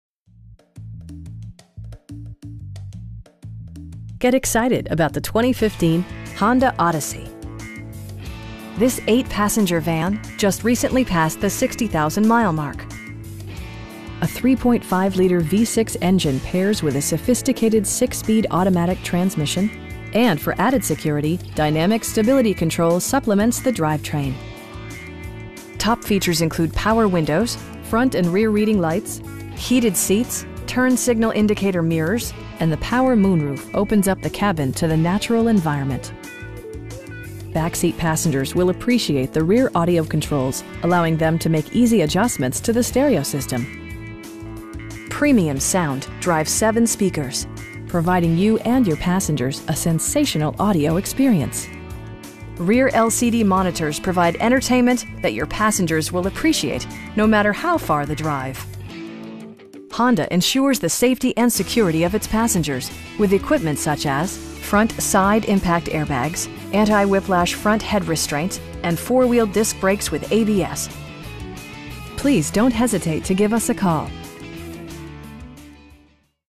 2015 Honda Odyssey Touring in Rock Hill, SC 29730
Honda Cars of Rock Hill
686 Galleria Boulevard

Get excited about the 2015 Honda Odyssey. This 8 passenger van just recently passed the 60,000 mile mark! A 3.5 liter V-6 engine pairs with a sophisticated 6 speed automatic transmission, and for added security, dynamic Stability Control supplements the drivetrain. Top features include power windows, front and rear reading lights, a power seat, heated seats, turn signal indicator mirrors, a power rear cargo door, remote keyless entry, and the power moon roof opens up the cabin to the natural environment. Features such as automatic climate control and leather upholstery prove that economical transportation does not need to be sparsely equipped. Storage solutions are integrated throughout the interior, demonstrating thoughtful attention to detail. Back seat passengers will appreciate the rear audio controls, allowing them to make easy adjustments to the stereo system. Premium sound drives 7 speakers, providing you and your passengers a sensational audio experience. Rear LCD monitors provide entertainment that your passengers will appreciate no matter how far the drive. Honda ensures the safety and security of its passengers with equipment such as: dual front impact airbags, front side impact airbags, traction control, brake assist, anti-whiplash front head restraints, a panic alarm, and 4 wheel disc brakes with ABS. This vehicle has achieved Certified Pre-Owned status, by passing Honda's comprehensive certification process, including an exhaustive 150-point inspection! Our aim is to provide our customers with the best prices and service at all times. Please don't hesitate to give us a call.